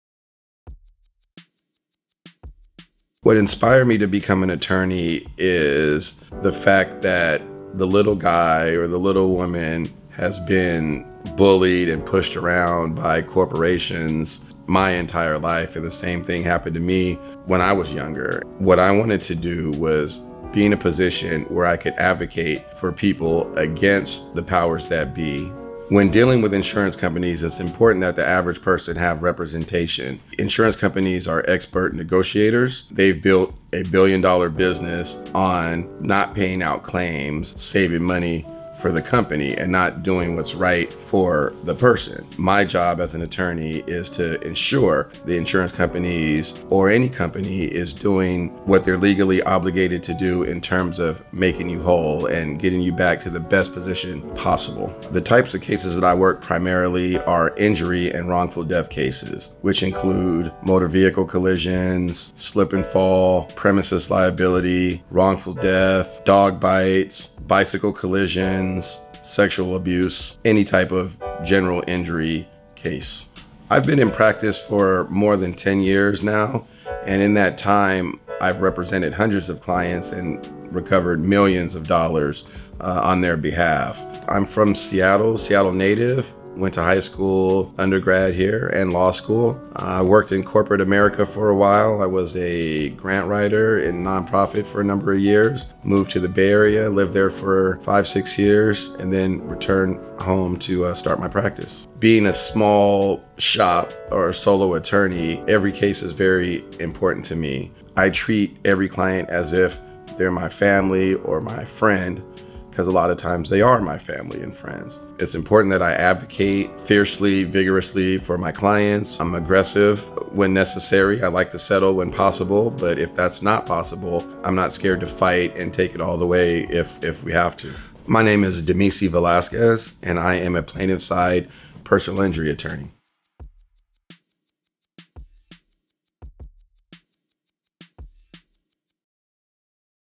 Damisi Velasquez Attorney At Law | The 206 Lawyer | Seattle, WA
I’m a Seattle native and a plaintiff’s side lawyer at heart.  I understand how emotional dealing with these situations can be. I encourage you to focus on getting treatment while I work tirelessly on recouping your losses to get you maximum compensation to the full extent allowed by law.

Big, complex cases involving personal injury, discrimination or abuse require a seasoned lawyer who has seen it all. I know how to deal with the multiple parties involved and press the right buttons to get the results we need.

As a boutique firm, my staff and I are able to provide the personal attention each case deserves. Clients appreciate my candor, diligence and proactive communication to keep their case moving forward.

1700 7th Avenue, Suite 2280